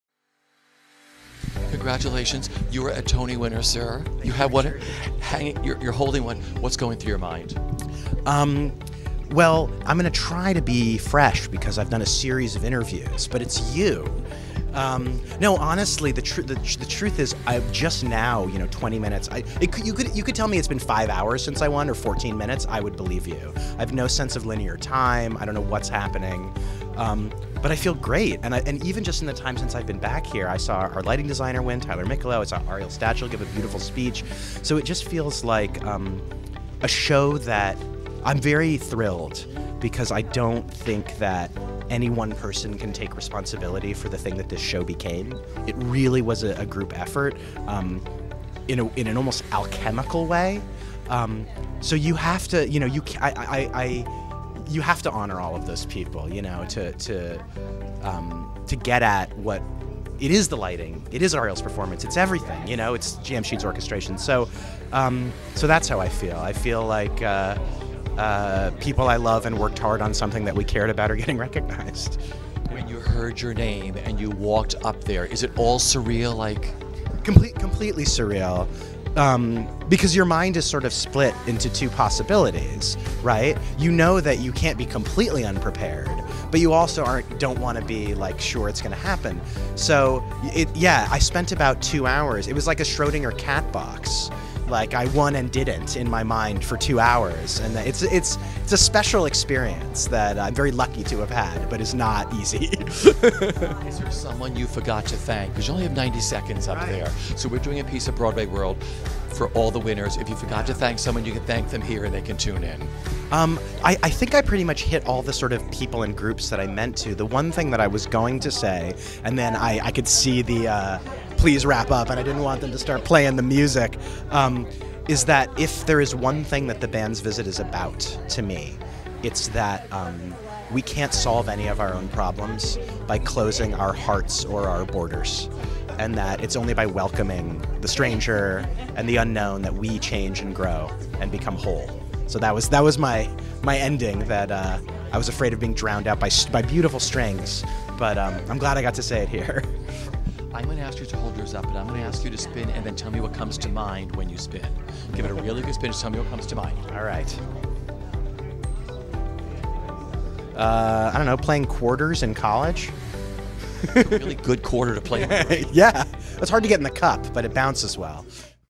Tony Talk: Best Book of a Musical, Itamar Moses
From BroadwayWorld's Archive: Itamar Moses has won the 2018 Tony Award for Best Book of a Musical for THE BAND'S VISIT.